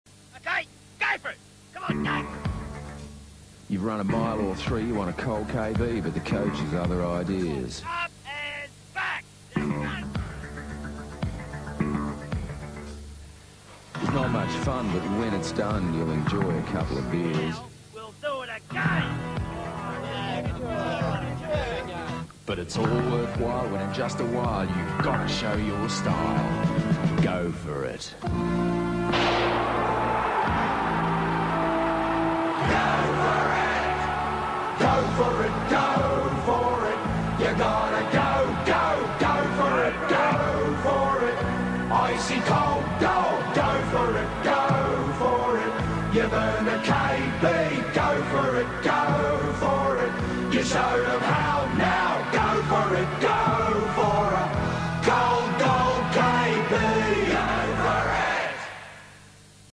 KB Beer Ad (Australian TV Commercial from mid 1980s)
This ad was on Australian TV in the mid 1980s.
My dad, Derek Moritz, was the coach.
Please excuse the counter on the screen, but I can't get it off my old video player (the remote control broke)!